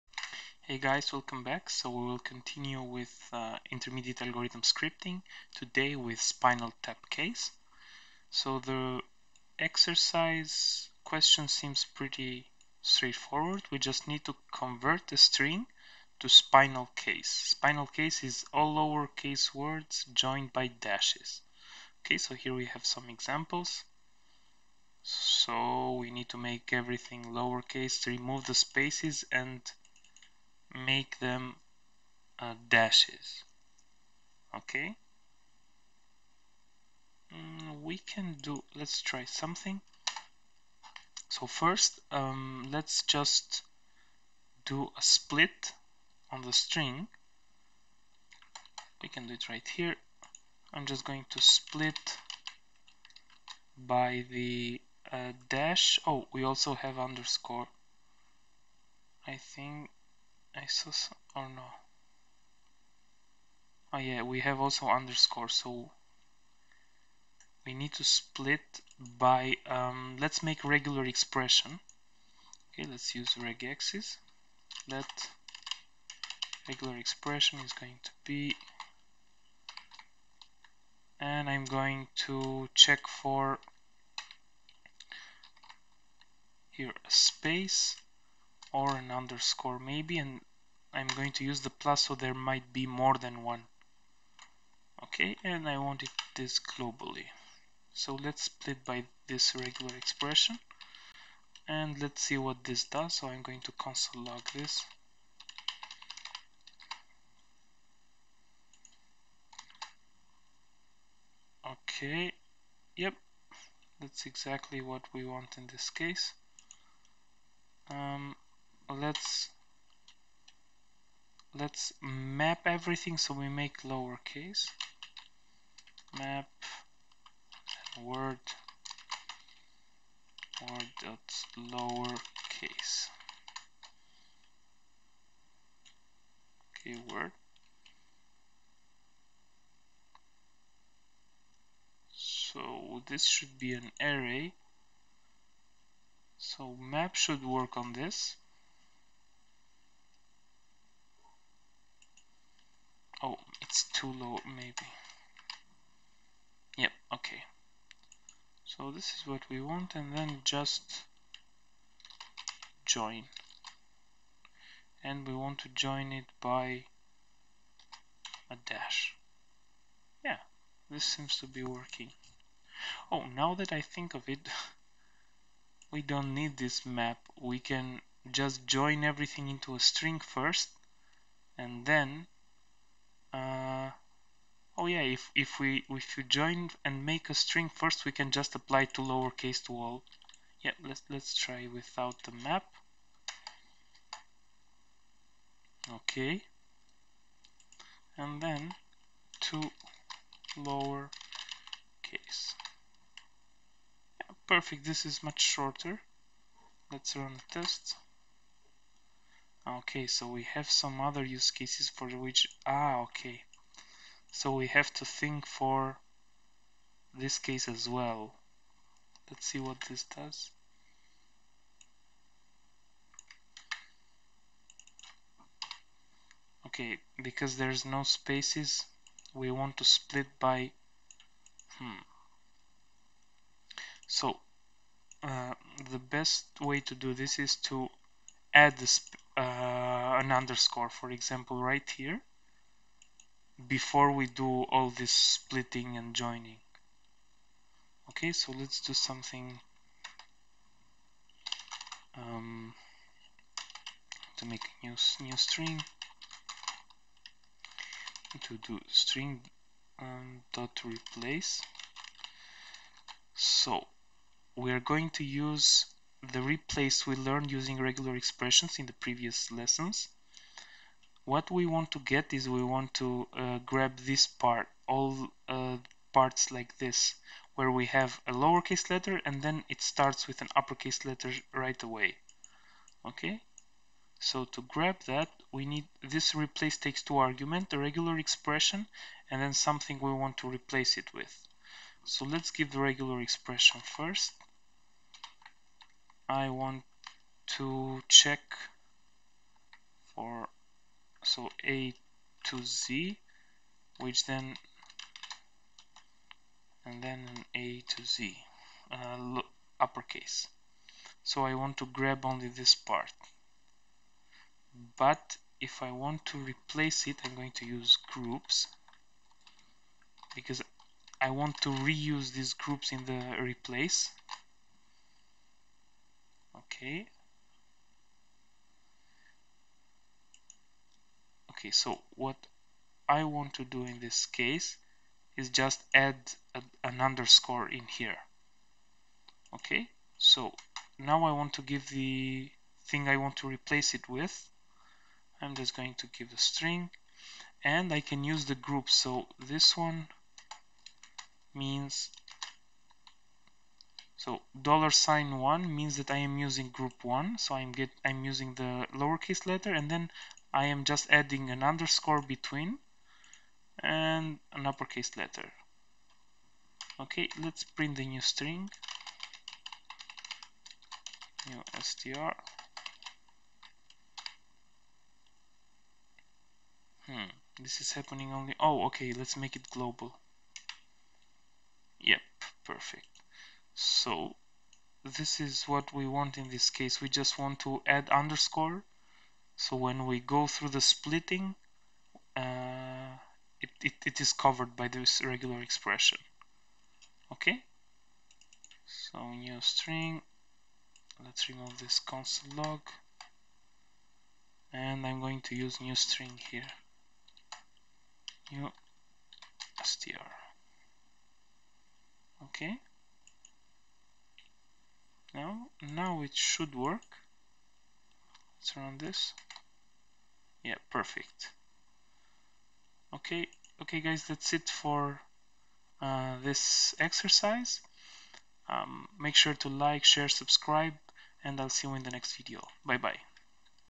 freeCodeCamp solutions - Spinal Tap Case
In this video series I am going to go through the exercises in the Freecodecamp's curriculum, for JavaScript Algorithms and Data Structures: